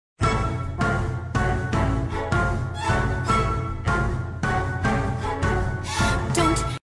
Adding perfume to your laundry to make it smell good
Adding perfume to your laundry

That’s a no-no

Hot water and laundry detergents break down odour molecules, so you won’t smell your perfume.

Also, oil in the fragrance can stain your clothes permanently.